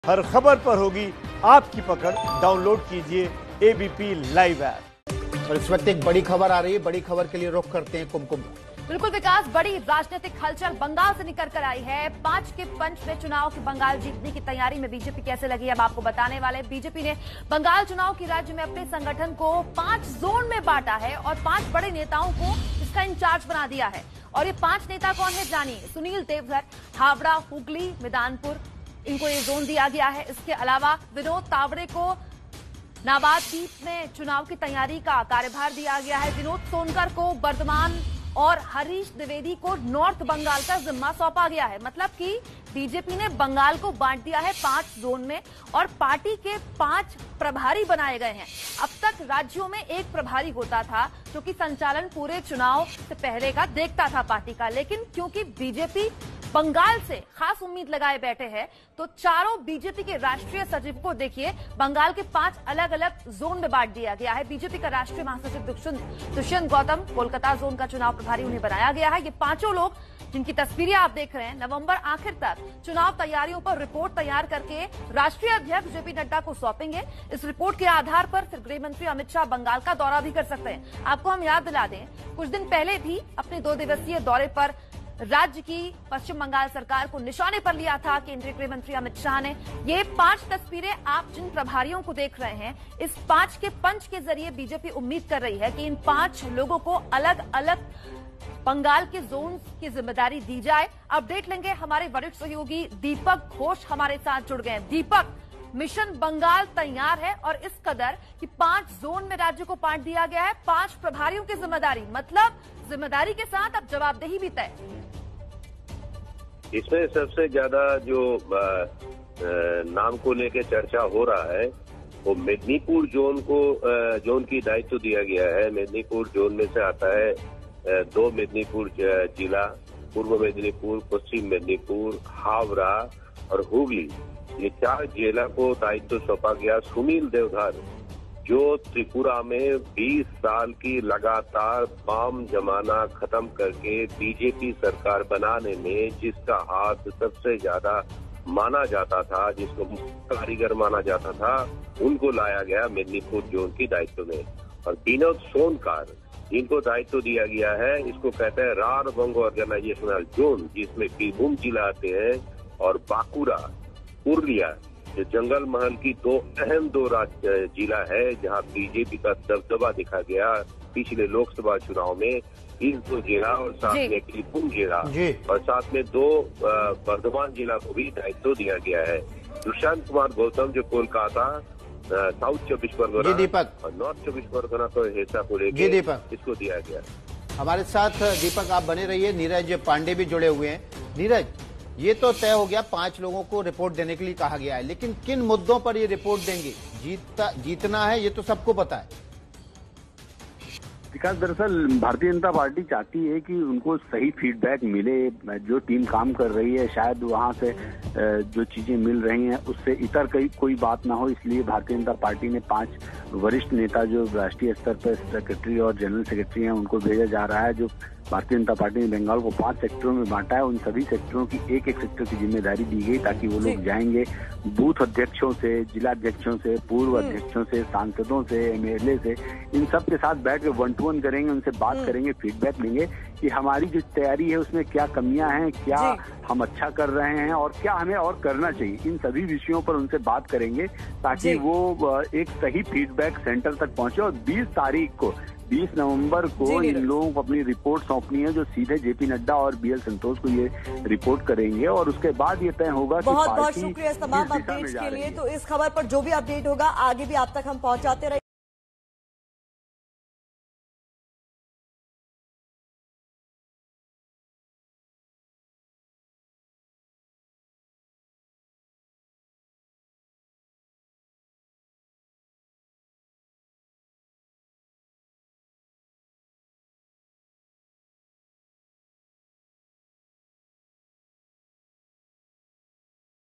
West Bengal विजय के लिए BJP ने 5 नेताओं की अगुवाई में बनाई पांच टीमें | ABP News Hindi
बिहार के बाद बंगाल विजय के लिए बीजेपी ने पांच नेताओं की अगुवाई में बनाई पांच टीमें, नवंबर के अंत तक पार्टी अध्यक्ष को देंगे रिपोर्ट.